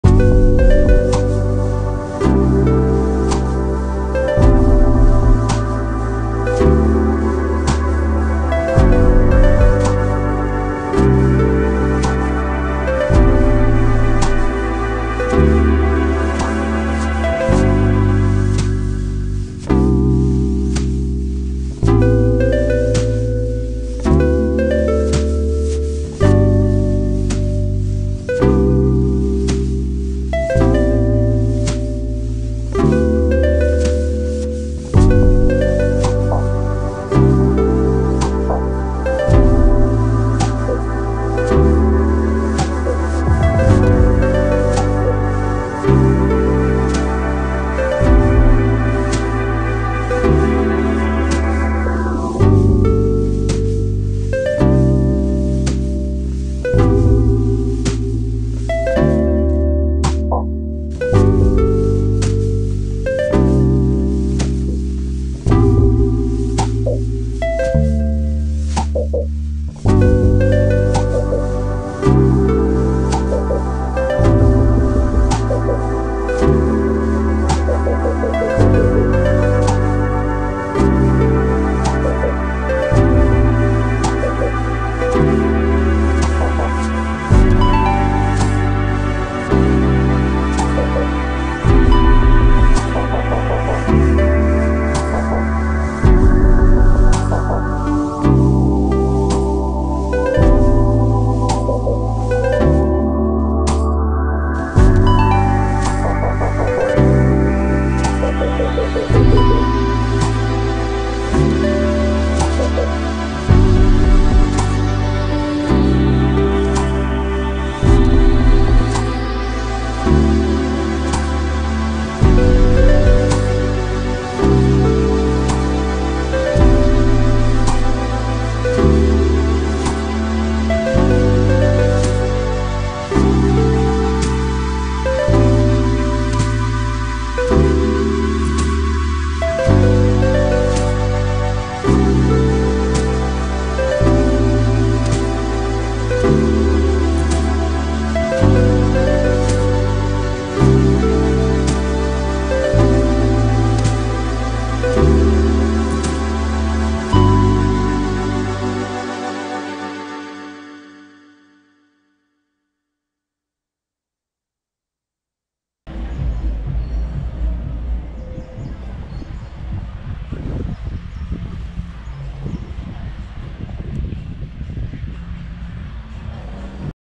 Hotel RIU PALACE OASIS ***** 🏨 | Gran Canaria | Room
Room at Riu Palace Oasis Hotel in Gran Canaria (Spain).

Habitación en el Hotel Riu Palace Oasis en Gran Canaria (España).